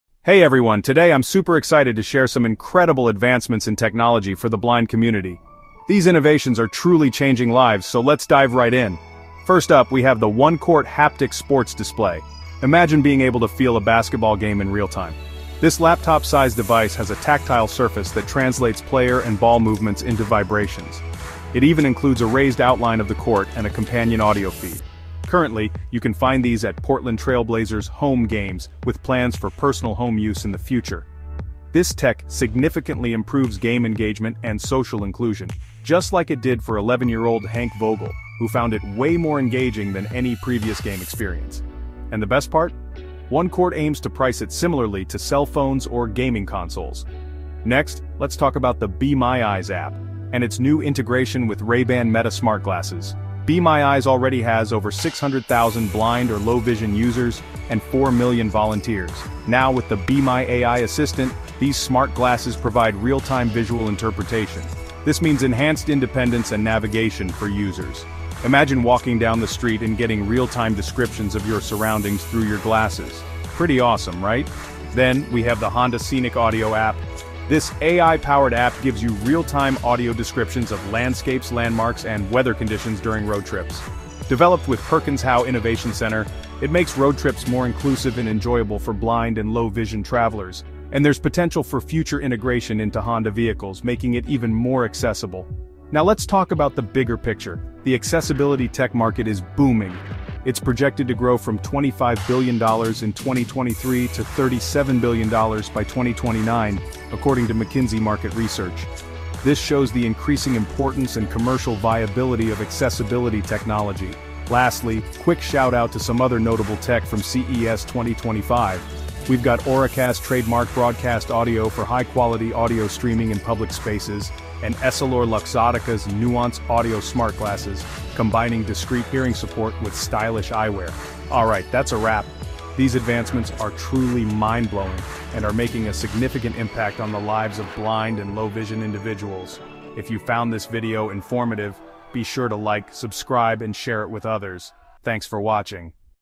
Tech Revolution for the Blind: Game-Changing Innovations!
Explore the latest game-changing innovations that are revolutionizing the lives of the visually impaired! From AI-powered smart canes to virtual reality experiences, we're diving into the cutting-edge technology that's empowering the blind community. Join us as we uncover the tech revolution that's breaking down barriers and opening up new possibilities for the visually impaired. Get ready to be inspired by the incredible stories of resilience, innovation, and progress!